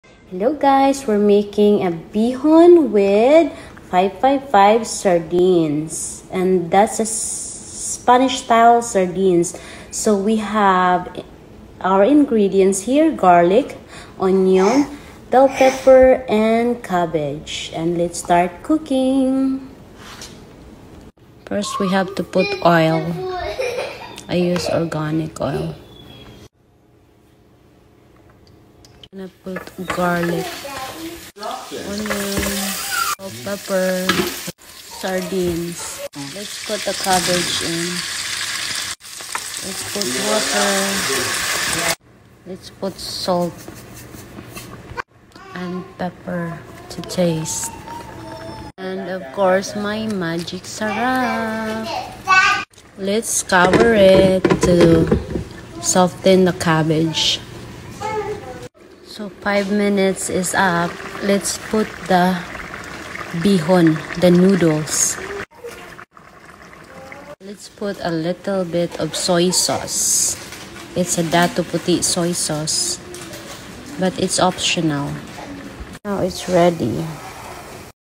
Bihon with Spanish Style Sardines | Pinoy Food | Simple and easy but so delicious
Ingredients
•coocking oil
•garlic
•onion
•bell pepper
•Spanish Style Sardines with Corn Oil
•bihon or Rice noodles
•salt
•ground black pepper
•soysauce